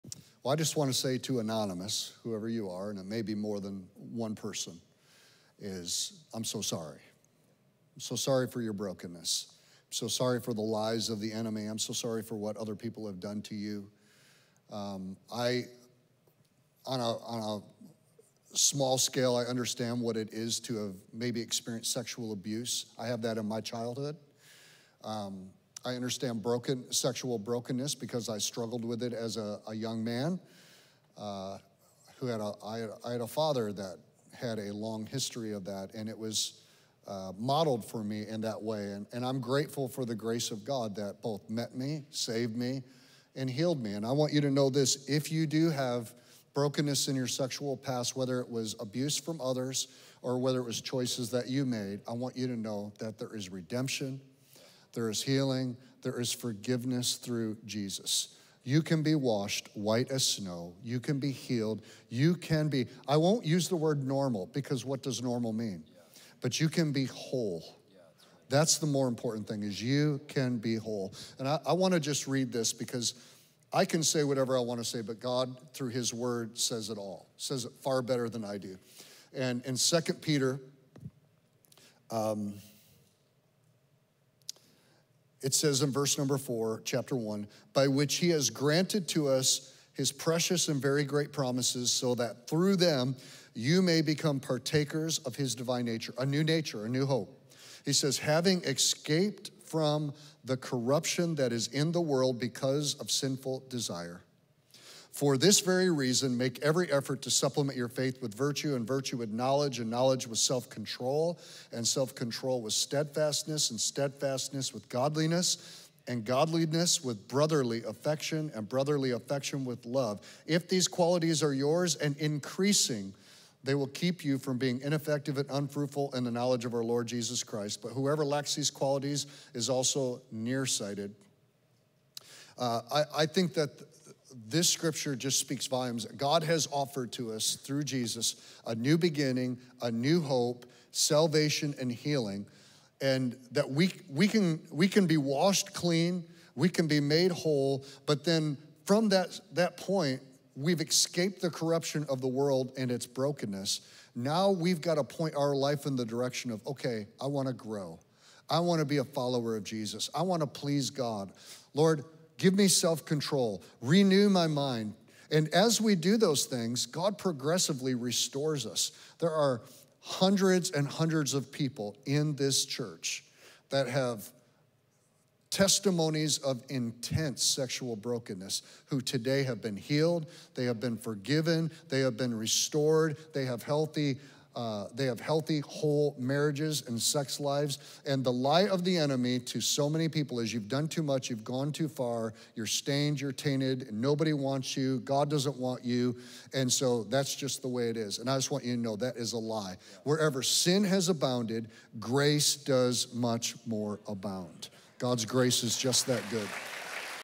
I've Faced Sexual Brokenness and Abuse. How Do I Not Let My Past Wounds Define Me? | Red Hot
This video is from Radiant Church's RED HOT series, an unscripted time with Pastor Lee Cummings where you can ask your burning questions and get immediate, unprepared responses.

Through Jesus, God offers redemption, healing, and forgiveness for past wounds like sexual abuse, enabling individuals to be washed white as snow and become whole rather than being defined by their past.

Second Peter 1:4 outlines God's promises through Jesus, allowing believers to partake in divine nature, gain new hope, escape worldly corruption, and grow in virtues like faith, self-control, and love.

Hundreds of people at the speaker's church have testimonies of overcoming intense sexual brokenness, achieving healing, forgiveness, and restoration, leading to healthy marriages and sex lives.

The enemy's lie that past wounds make one stained, tainted, and unwanted is countered by God's abundant grace, which offers complete transformation and renewal.